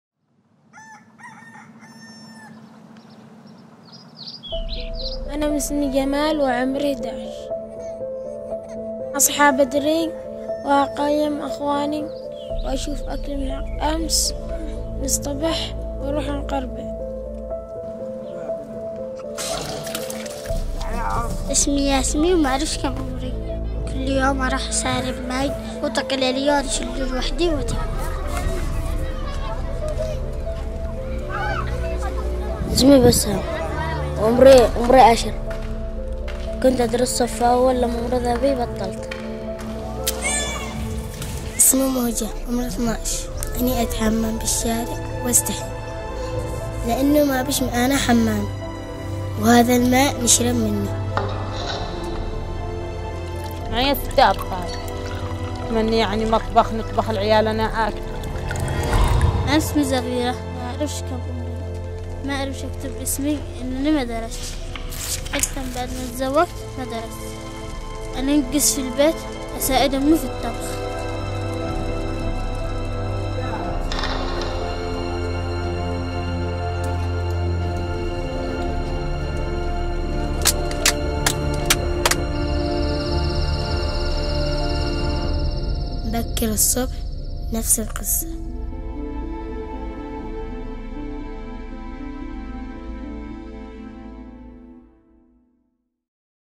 Our life - Yemen's forgotten children
UNICEF  - all rights are reserved 2017©
A day in the life of Yemen's forgotten children

˚dotnotion media production

Sana’a - Yemen, Hadda St. ,Almusbahi round, KIA car show- room building, 2nd  floor.